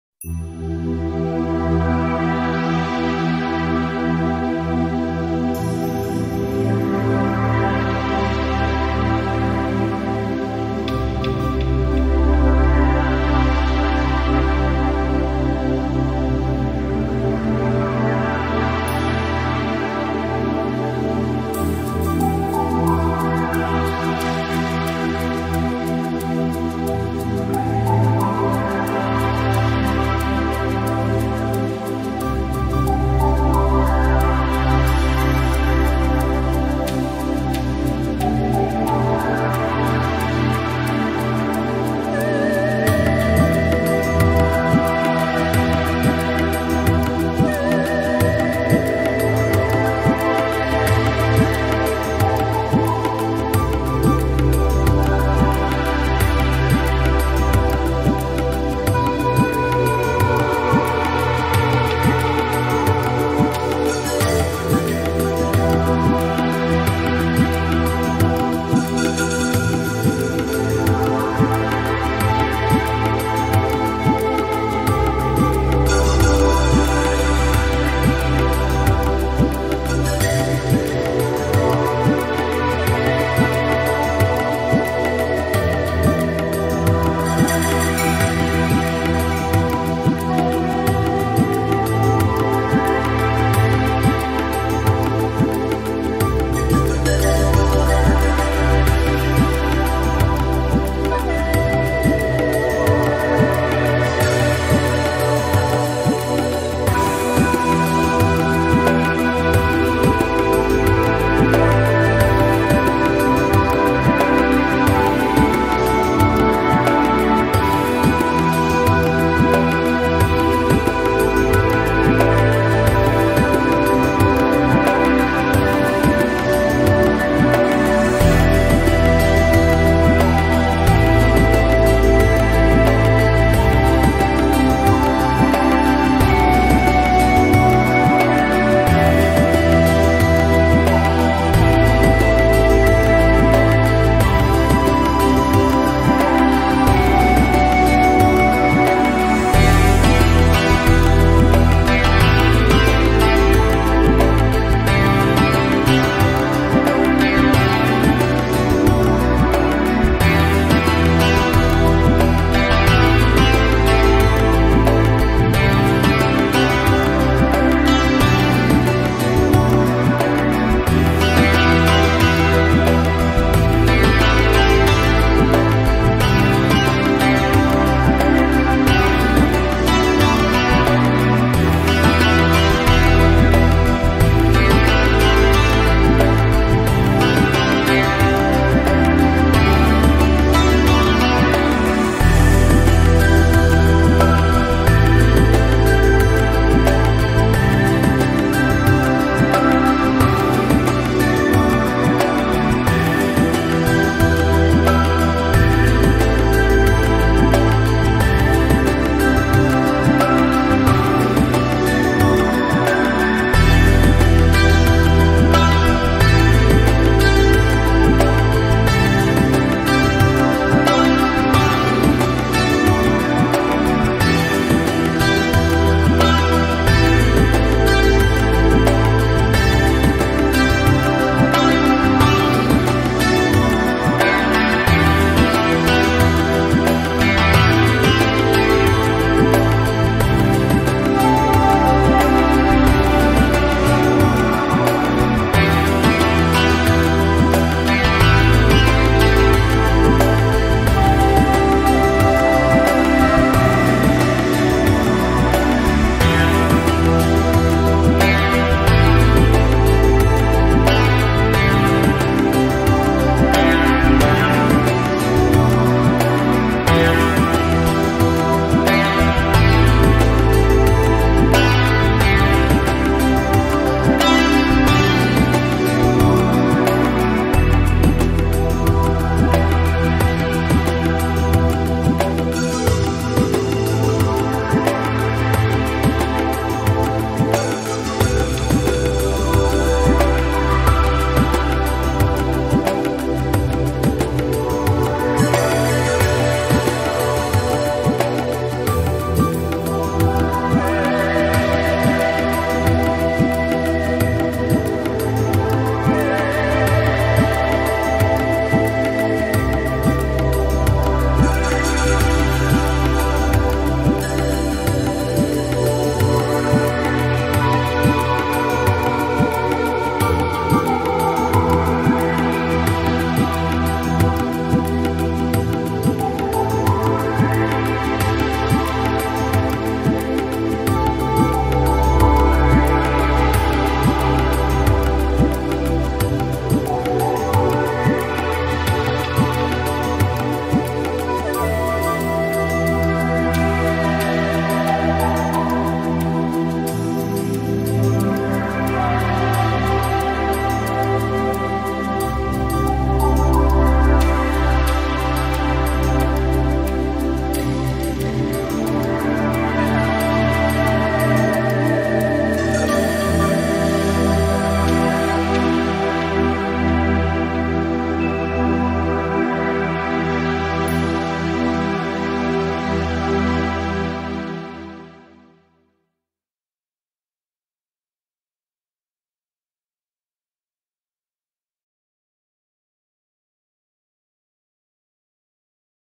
Relaxation | Peace | Calm
Relaxing music that heals stress, anxiety and depression, heals the mind.
Music for relaxation, meditation, study, reading, massage, spa. This music is perfect for dealing with anxiety, stress as it promotes relaxation and helps eliminate bad vibes.